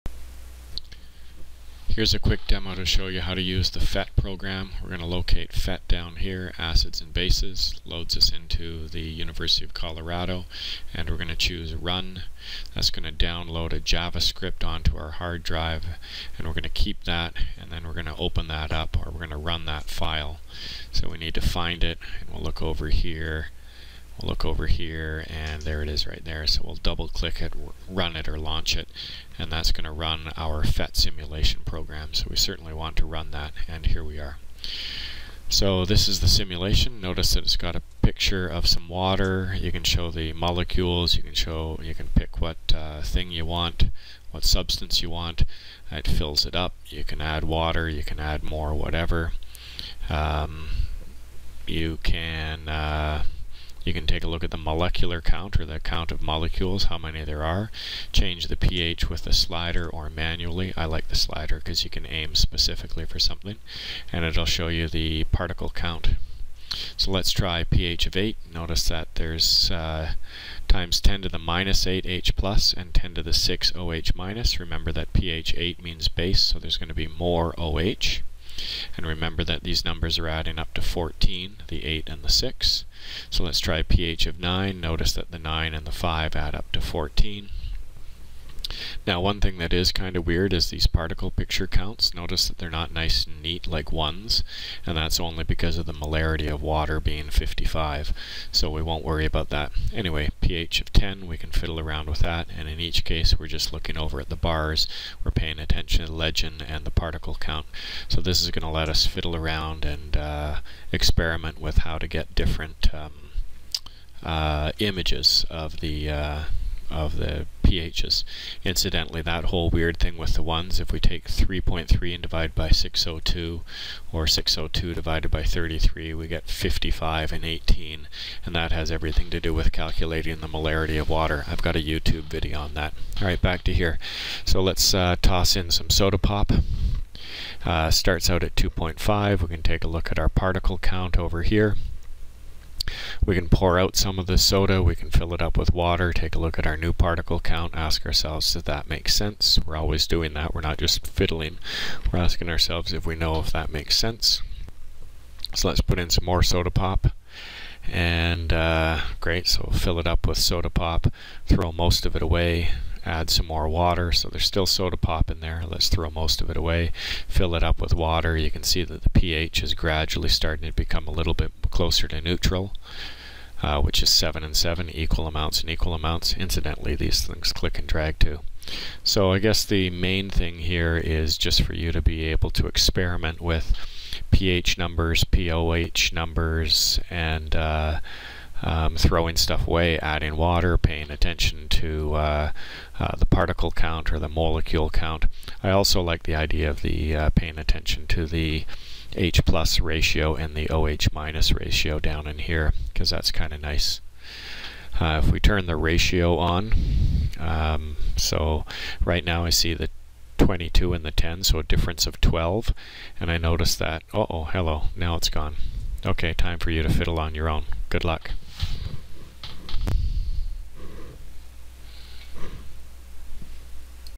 Using PHET's Acid and Base pH Simulation
a quick overview of how to play with and experiment with University of Colorado's PHET simulations for acids and bases and pH.